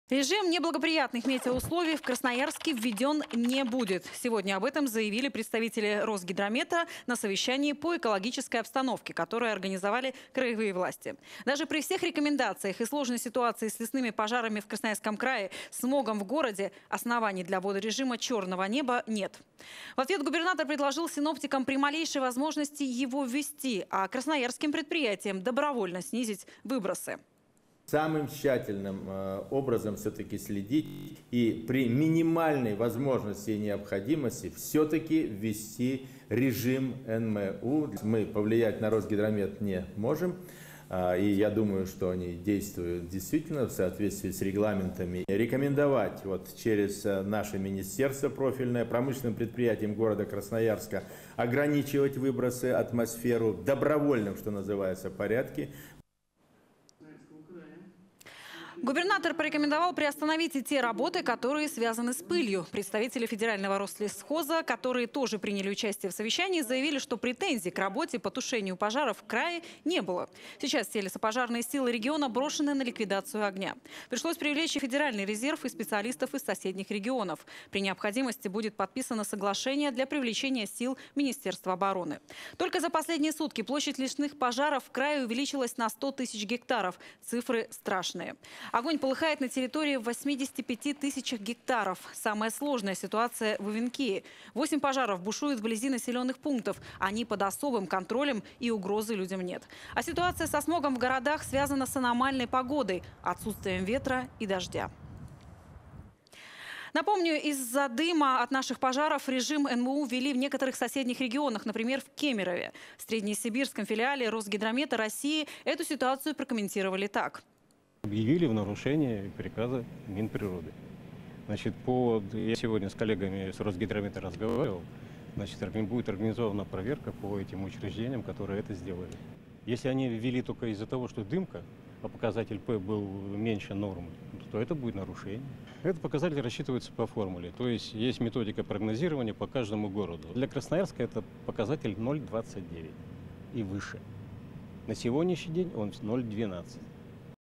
Режим НМУ введен не будет
Режим неблагоприятных метеоусловий в Красноярске введен не будет. Сегодня об этом заявили представители Росгидромета на совещании по экологической обстановке, которое организовали краевые власти. Даже при всех рекомендациях и сложной ситуации с лесными пожарами в Красноярском крае, смогом в городе, оснований для ввода режима черного неба нет. В ответ губернатор предложил синоптикам при малейшей возможности его ввести, а красноярским предприятиям добровольно снизить выбросы.

Александр Усс, губернатор Красноярского края:

— Самым тщательным образом все-таки следить и при минимальной возможности и необходимости все таки ввести режим НМУ. Мы повлиять на Росгидромет не можем и, я думаю, что они действуют в соответствии с регламентами.

Губернатор порекомендовал приостановить и те работы, которые связаны с пылью. Представители федерального Рослесхоза, которые тоже приняли участие в совещании, заявили, что претензий к работе по тушению пожаров в крае не было. Сейчас все лесопожарные силы региона брошены на ликвидацию огня. Пришлось привлечь и федеральный резерв и специалистов из соседних регионов. При необходимости, будет подписано соглашение для привлечения сил министерства обороны. Только за последние сутки площадь лесных пожаров в крае увеличилась на 100 тысяч гектаров, цифры страшные. Огонь полыхает на территории в 850 тысяч гектаров. Самая сложная ситуации в Эвенкии. 8 пожаров бушуют вблизи населенных пунктов, они под особым контролем и угрозы людям нет. А ситуация со смогом в городах связана с аномальной погодой — отсутствием ветра и дождя.

Напомним, из дыма от наших пожаров режим НМУ ввели в некоторых соседних регионах, например, в Кемерове. В Среднесибирском филиале Росгидромета России эту ситуацию прокомментировали так.Сергей Серёжкин, и. о. начальника ФГБУ «Среднесибирского управления по гидрометеорологии и мониторингу окружающей среды»:

— Это нарушение приказа Минприроды, я сегодня с коллегами Росгидромета разговаривал, будет организована проверка по этим учреждениям, которые это сделали, если они ввели только из-за того, что дымка, а показатель был меньше нормы, то это нарушение, этот показатель рассчитывается по формуле, есть методика прогнозирования по каждому городу, для Красноярска этот показатель 0,29 и выше, на сегодняшний день он 0,12.